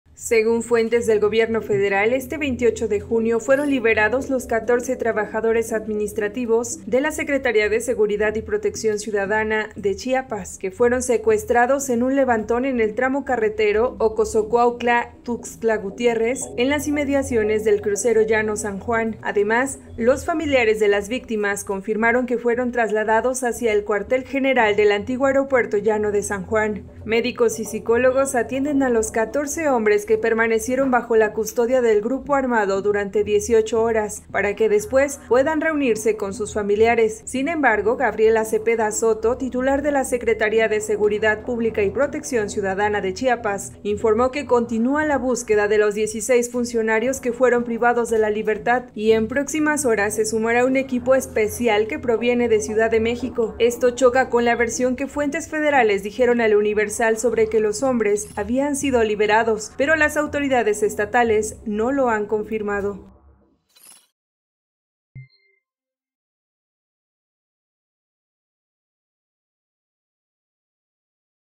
Liberan a trabajadores de Seguridad Pública secuestrados en Chiapas por comando armado
Los 14 empleados que permanecieron 18 horas secuestrados, son atendidos por médicos y psicólogos

¡Contesta esta encuesta y participa en el sorteo de tarjetas de regalo de 50 dólares!